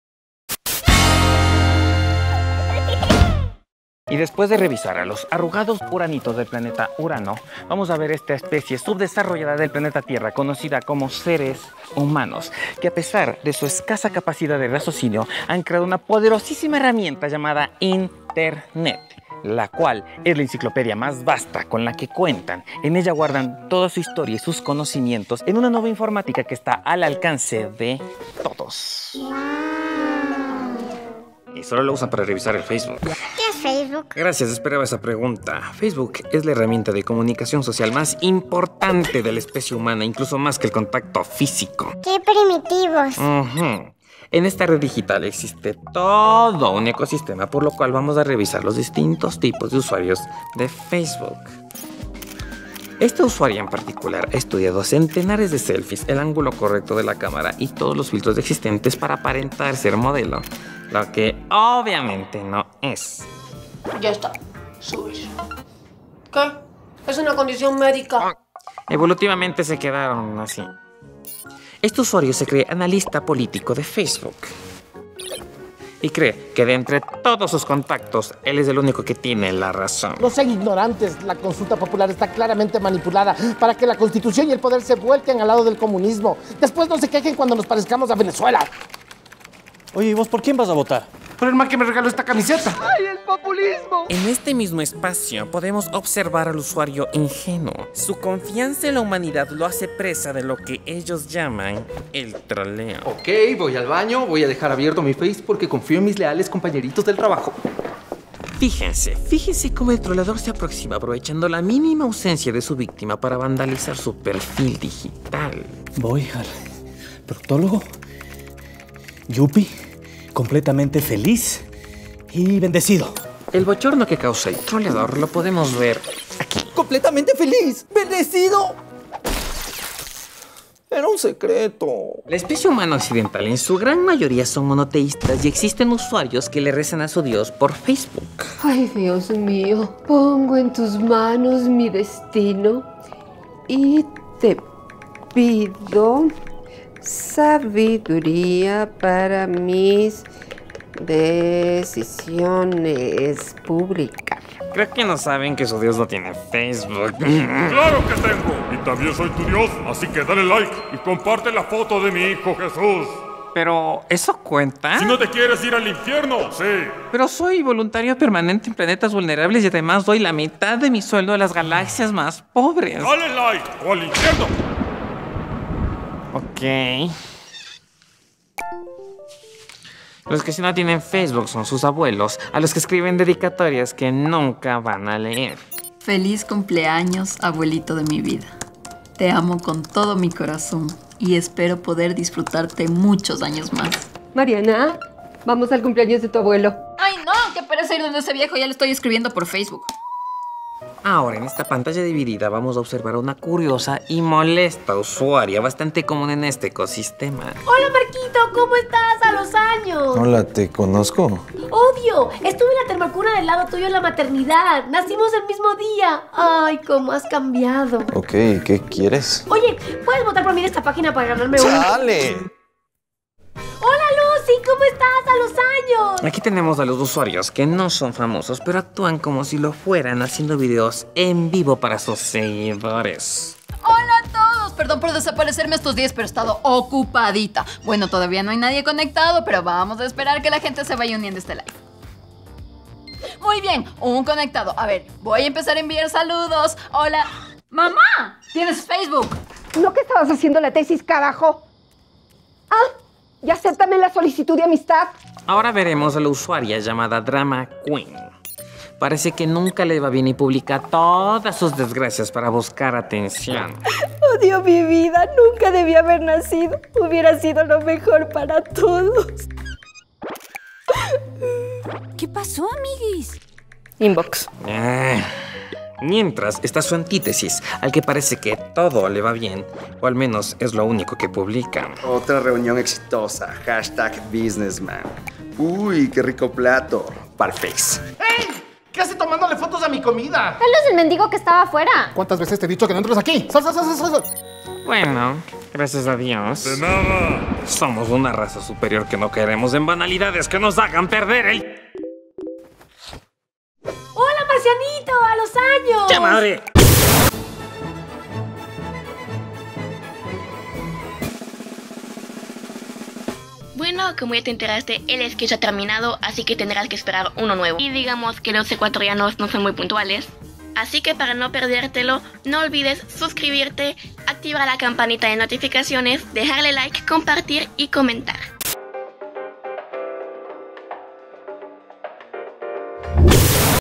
Tipos de Usuarios de Facebook
Este sketch está muy viejo, mejor mira los más recientes :P
Todo un ecosistema alrededor de una herramienta 🙄 ¡Qué primitivos!

Elenco:

Alien: Orlando Herrera;
Niño alienígena: Gabriel Saltos
Niña alienígena: Martina Pacheco
Darío: José Pacheco
Compañero Troll: Emilio Reyes
Lucho: Daniel Machado
Mariana: Carla Yepez
Mamá Mariana: Beatriz Vergara
Sandra: María Karla Gómez
Marco: Paúl Guerra
Drama Queen: Nataly Valencia
Miguel: Jorge Ulloa
Mesero: Cristhian Quintana

Crew:
Dirección: Jorge Ulloa
Gerente de Producción: Juan Carlos Flor
Asist. Dirección: Sebastián Luna
Coord. Producción: Cristobal D´Onofrio
Prod. de Campo: Stefanía Moya
Locacionista 1 y Asist. Producción: May Grijalva
Locacionista 2 y Asist. Producción: Andrea Zambrano
Dir. Fotografía: Alejando Chauvin
Script: Edwin Vargas
Sonido Directo: David Arciniegas
Gaffer: Edison Muñoz
Grip 1: Alex Achicanoy
Dir. Arte: Erick Ferro
Asist. Arte: Danilo Boada
Vestuario: Devra Salas
Asist. Vestuario: Xavier López
Maquillaje: Gabriela Pérez
Making off: Emilio Reyes
Transporte y logística: Marco Pilacuán y David Pilacuán

Posproducción:
Posproducción Visual: - Jonathan Montero - Felipe Granda - Victor Moya
Posproducción Sonora: - Víctor Herrera - Daniel Domínguez

Guión:
Orlando Herrera - Juan Aguilar - Nataly Valencia - Jaime Zambrano

Un video nuevo cada semana.